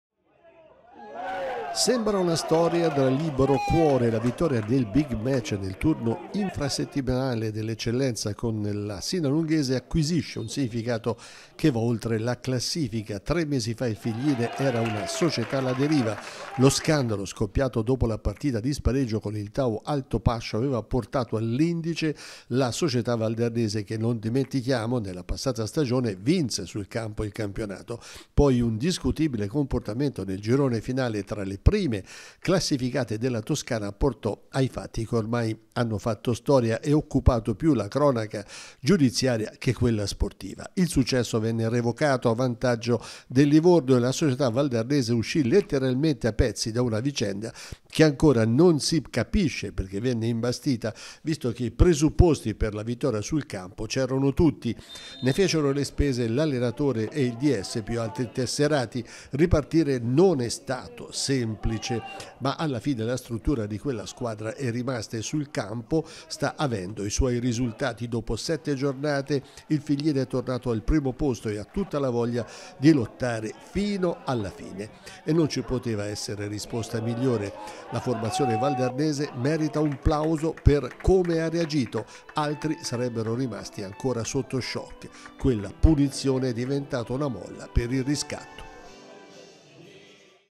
Eccellenza, il riscatto del Figline
Il successo nello scontro diretto con la Sinalunghese vale la conquista del primato nel girone B dell'Eccellenza. Il Figline si sta prendendo la rivincita dopo il ciclone che ha subito in estate.